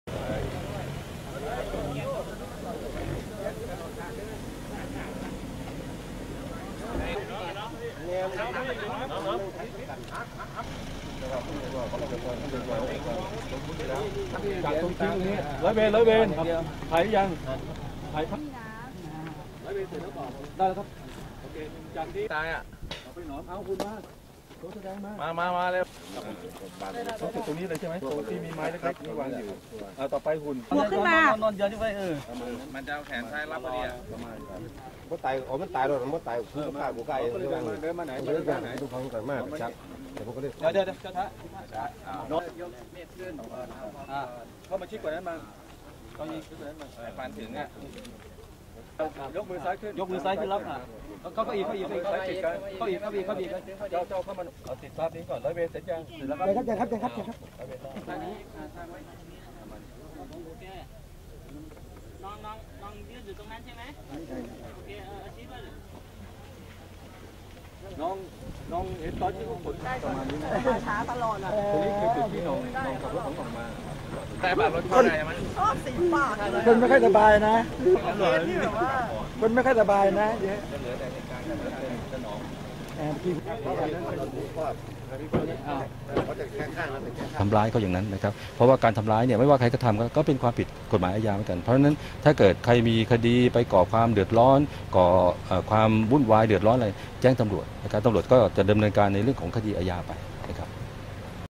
รวบแล้ว 2 ผู้ต้องหาฆ่า ”ป๊อปหน้าลาย” เผยผู้ตายทำตัวกร่าง-ข่มขืนผู้หญิงต้องใช้ศาลเตี้ยลงโทษ
2014/11/17 รวบแล้ว 2 ผู้ต้องหาฆ่า ”ป๊อปหน้าลาย” เผยผู้ตายทำตัวกร่าง-ข่มขืนผู้หญิงต้องใช้ศาลเตี้ยลงโทษ